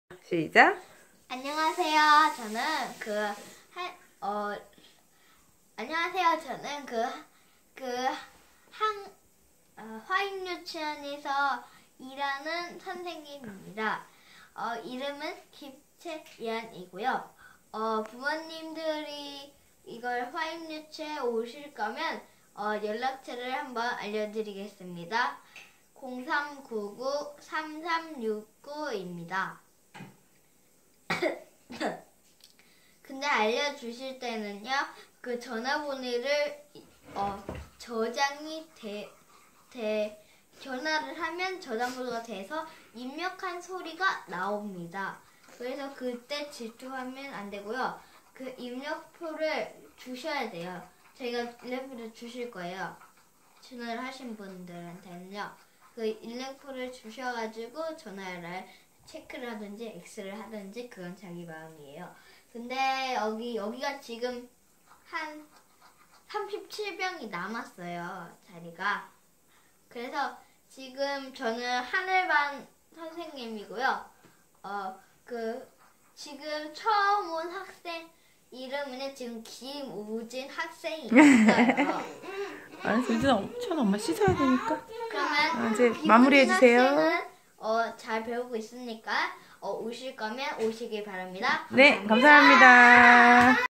저는 하늘반 선생님 김채연 입니다♡
채연이 유치원 선생님 놀이♡
아구아구 이뻐요♡